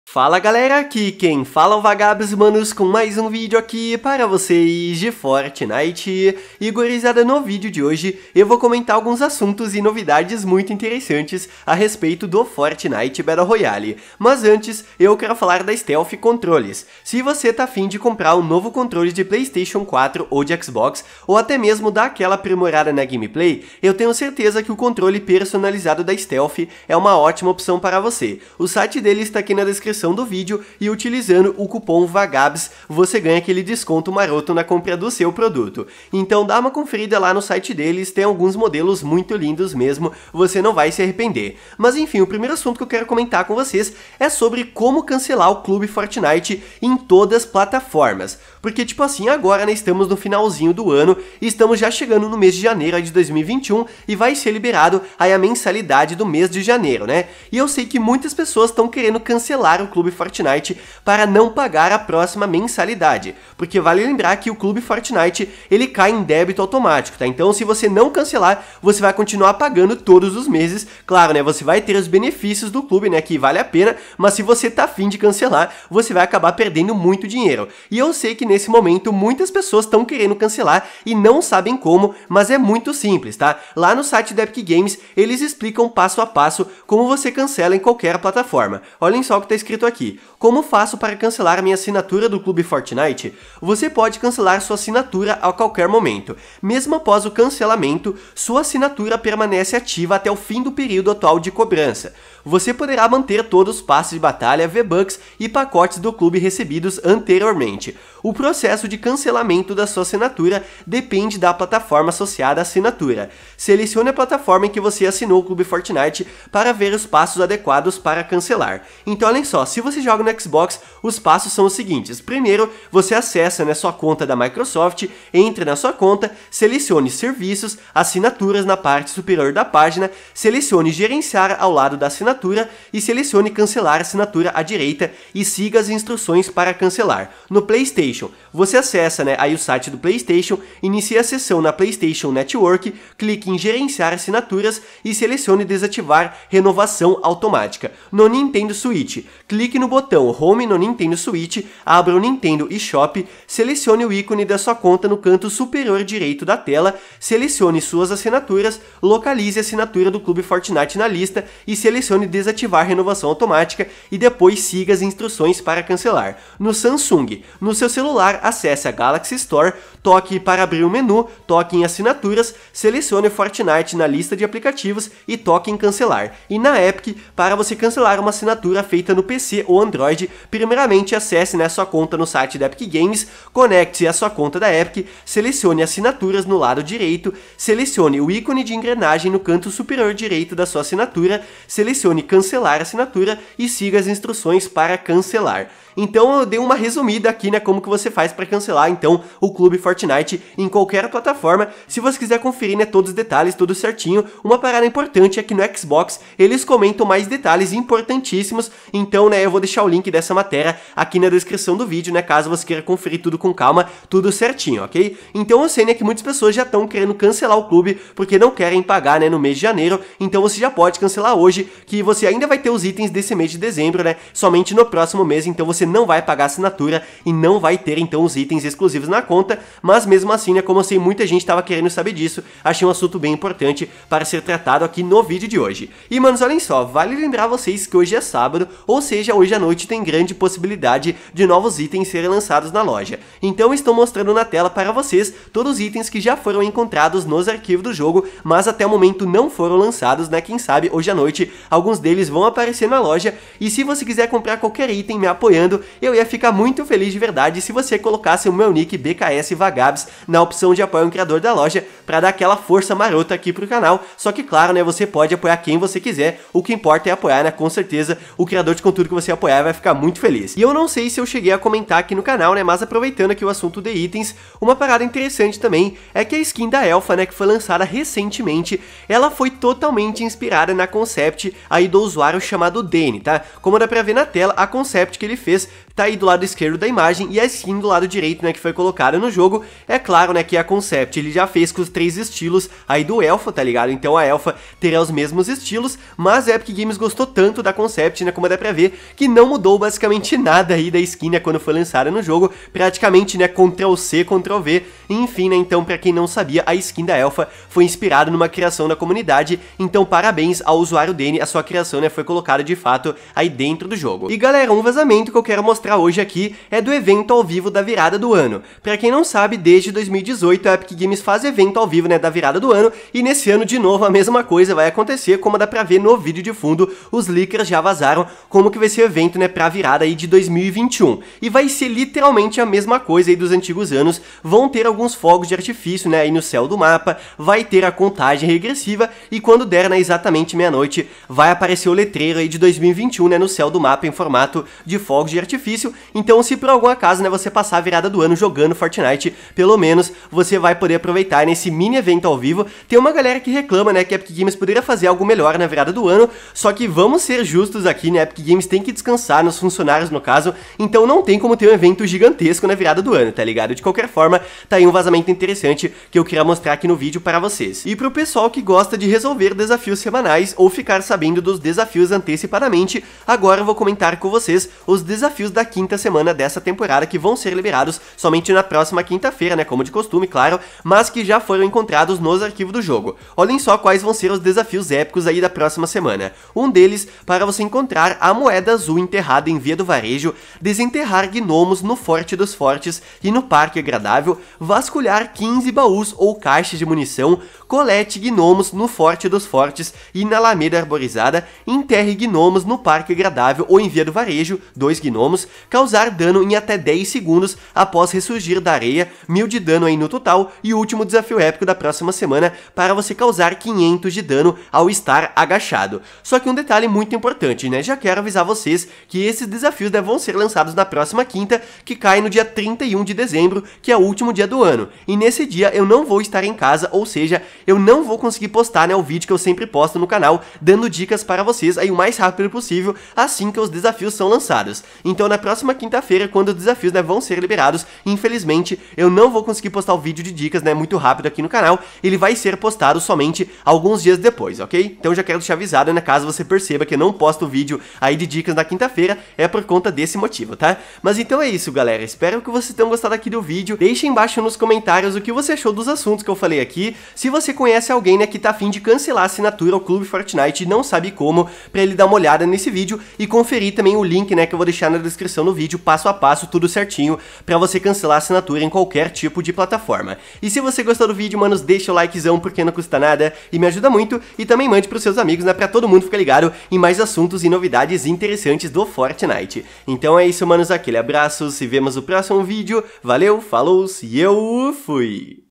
FORTNITE - CANCELAR CLUBE ASSINATURA e 2021 EVENTO AO VIVO?
Fala gurizada, nesse vídeo comento novidades do Fortnite Battle Royale, sobre evento ao vivo 2021, como cancelar assinatura Clube Fortnite, desafios da semana 5 e mais. Espero que gostem :)